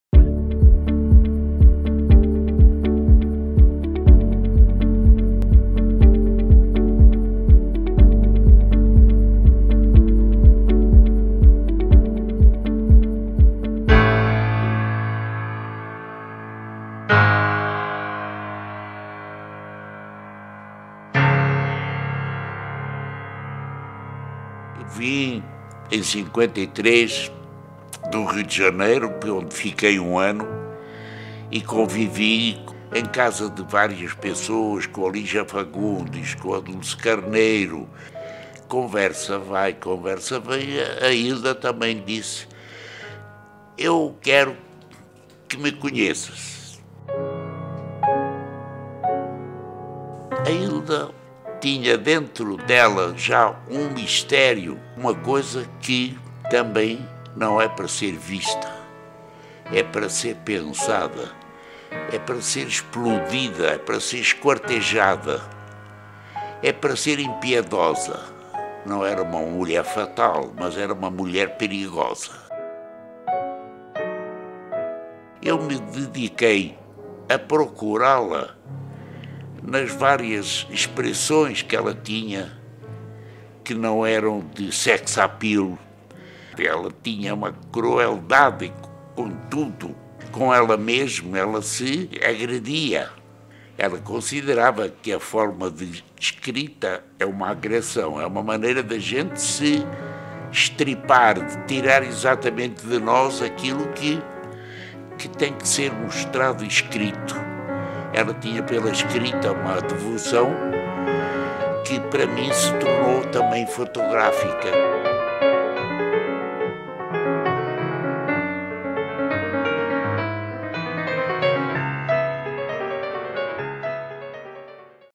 Fernando Lemos Hilda Hilst
O livro conta com o primeiro ensaio fotográfico feito por Fernando Lemos no Brasil, logo após sua chegada de Portugal, com a poeta Hilda Hilst. Um ensaio de Augusto Massi comenta esse encontro, somando-se a colagens feitas por Fernando sobre as fotos originais. O ensaio explora a trajetória de Fernando como poeta e fotógrafo, a produção de Hilda como jovem poeta e o encontro entre os dois.

O título está à venda nas livrarias, nas lojas das unidades do Sesc São Paulo e no